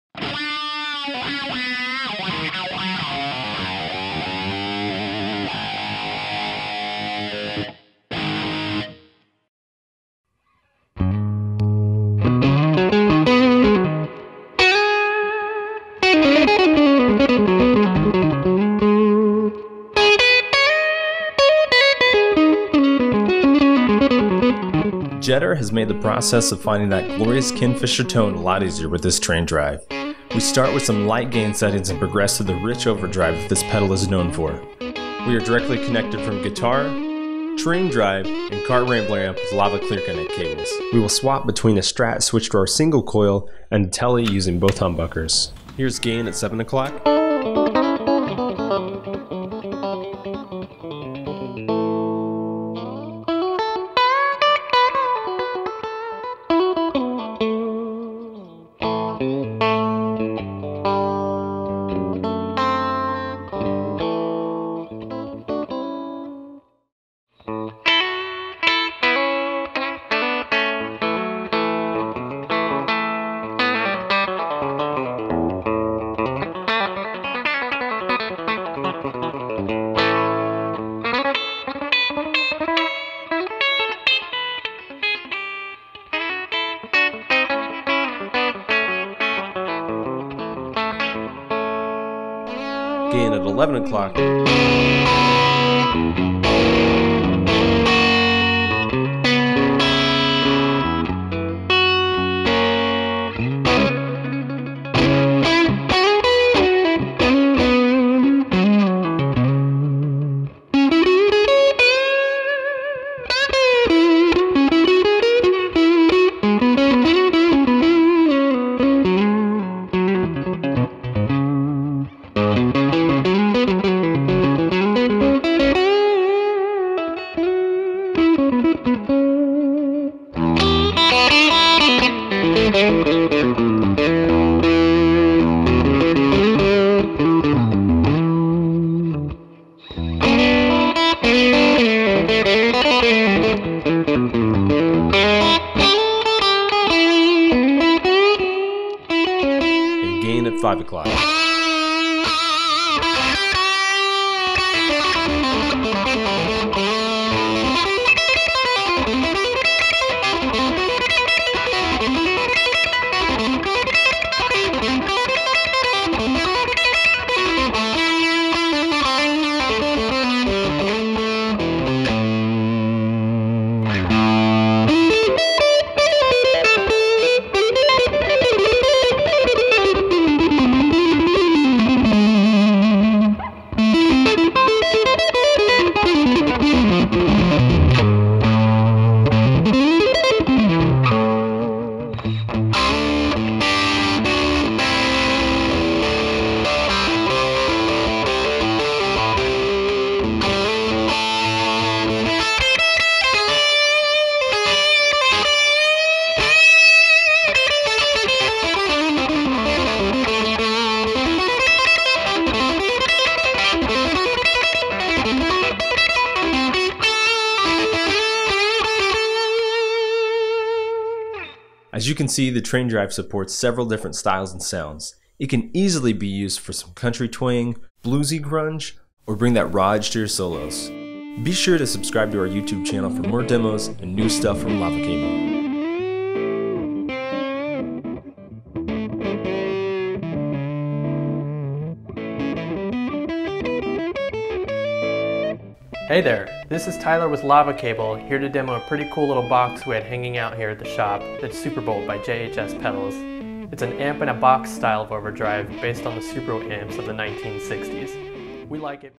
Jetter Traindrive Overdrive pedal demo
In this episode of 2 Toned Takes we look at the Jetter Traindrive Overdrive pedal.
It is the latest version of Jetter's line of "amp in a box" style pedals.
This particular pedal is designed to be as close as possible to one of Ken Fischer's highly famed Trainwreck amps.

To demo this pedal we used a SLJ Strat with single coils and switched back to a Tele with
humbuckers to give you a much better idea of how this would sound with your specific setup.
We ran Lava Clear Connect cable from the guitar to the pedal and from the pedal to the amp.
We kept the same settings on our Carr Rambler amp(Fender style amp) and used
three different gain settings on the pedal.

This was all recorded through a Shure SM 57 Mic through our Lava Fountain cable to a Tascam DR-40 Recorder.
Only the volume was increased in post on the guitar.
The amp setting were basically middle range except for Reverb that was at 11 O clock.